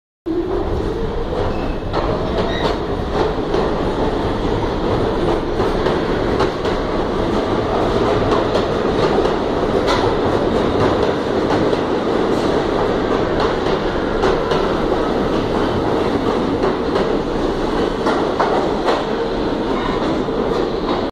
R46 R departing Whitehall street - South Ferry
looking around for some friends and got some footage while doing so